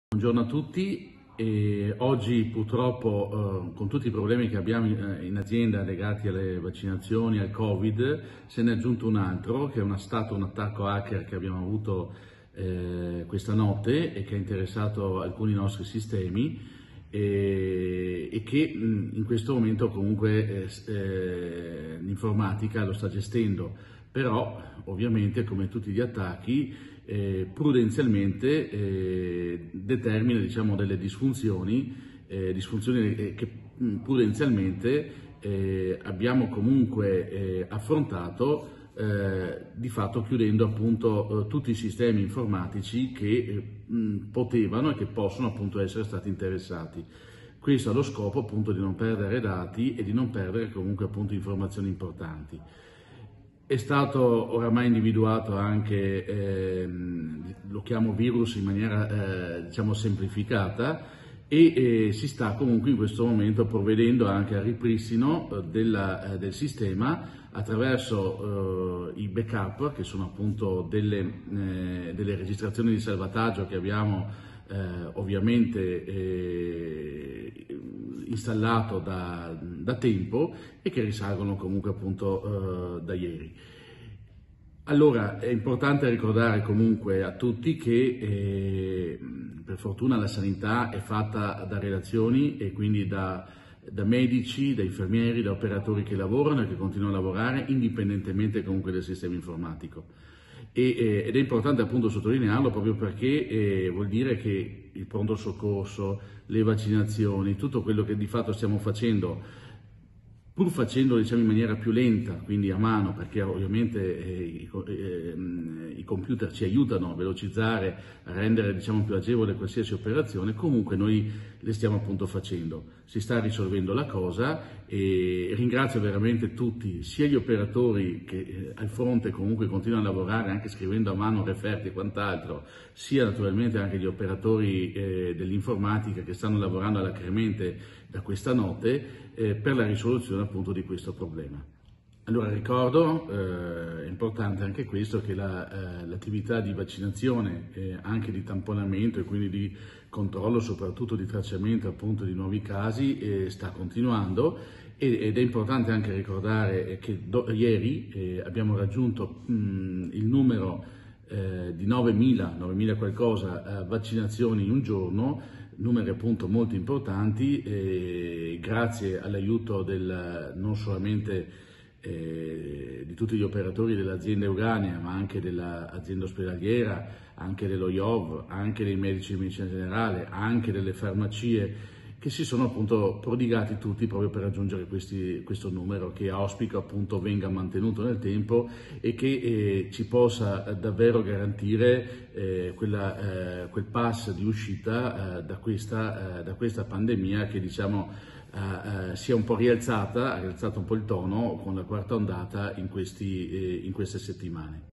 PARLA IL DIRETTORE GENERALE
Paolo Fortuna spiega cosa è successo al sito dell'Ulss6 Euganea, oggetto di un attacco hacker.